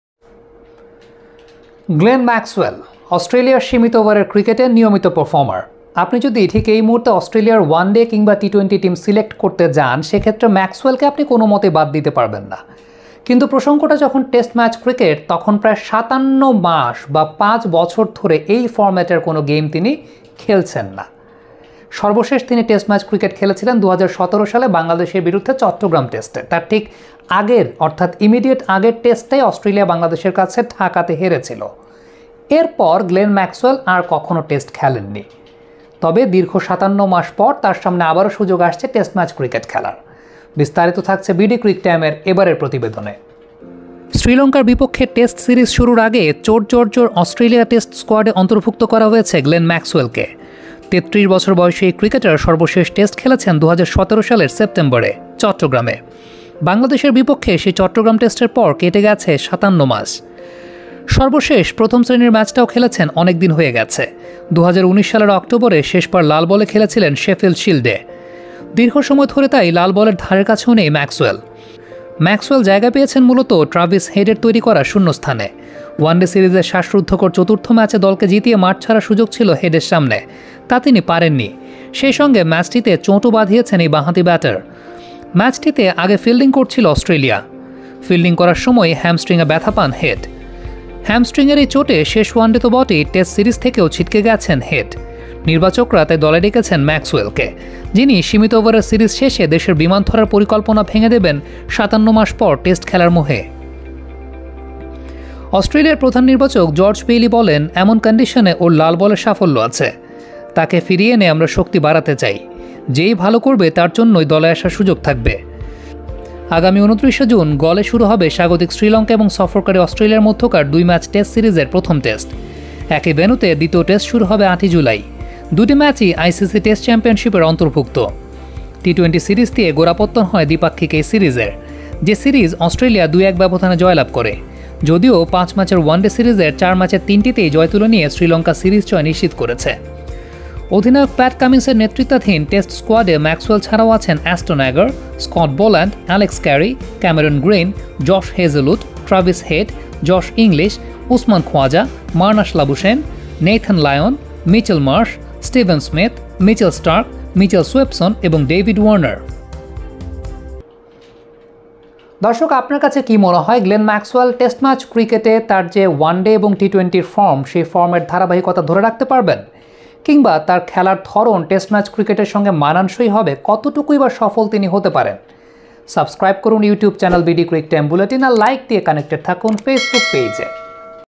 চট্টগ্রাম টেস্টের ৫৭ মাস পর টেস্ট খেলার দ্বারপ্রান্তে ম্যাক্সওয়েল || Glenn Maxwell || Australia
অস্ট্রেলিয়ার সীমিত ওভারের দলের কথা গ্লেন ম্যাক্সওয়েলকে ছাড়া ভাবা যাবে? ওয়ানডে কিংবা টি-টোয়েন্টি ম্যাক্সওয়েল দারুণ কার্যকরী এক অলরাউন্ডার। তবে টেস্টের আঙিনায় তার পা পড়ছিল না বললেই চলে। সেই ম্যাক্সওয়েল দীর্ঘ সময় পর টেস্ট খেলার দ্বারপ্রান্তে, যিনি সর্বশেষ টেস্ট খেলেছিলেন ২০১৭ সালে বাংলাদেশের মাটিতে। অস্ট্রেলিয়ার বিপক্ষে টাইগারদের পাওয়া ঐতিহাসিক ঢাকা টেস্ট জয়ের পর চট্টগ্রাম টেস্টে শেষবার ম্যাক্সওয়েলের গায়ে উঠেছিল অস্ট্রেলিয়ার টেস্ট জার্সি।